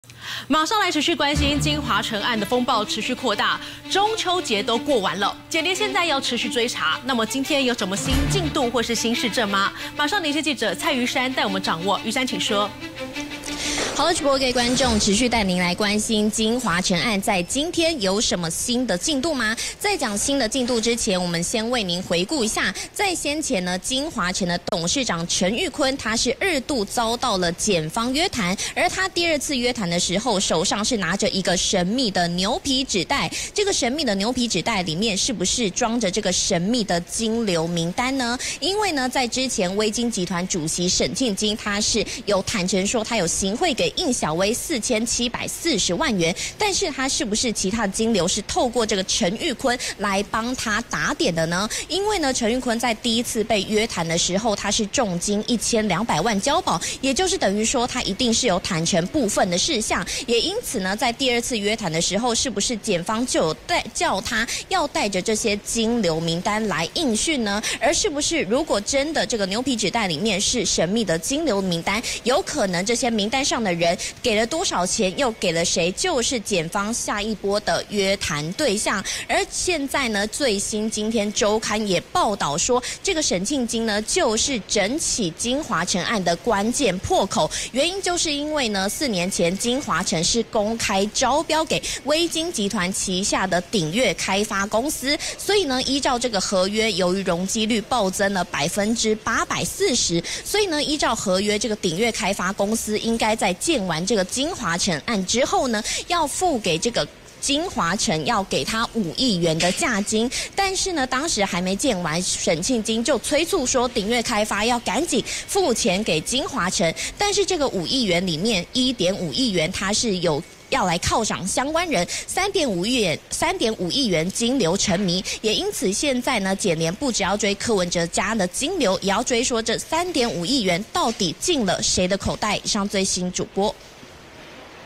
最新》柯文哲收押第14天！ 檢廉續追柯家關鍵金流
【無罪推定】

京華城案風暴持續擴大，中秋節後檢廉持續追查，今天有什麼新進度或新事證嗎？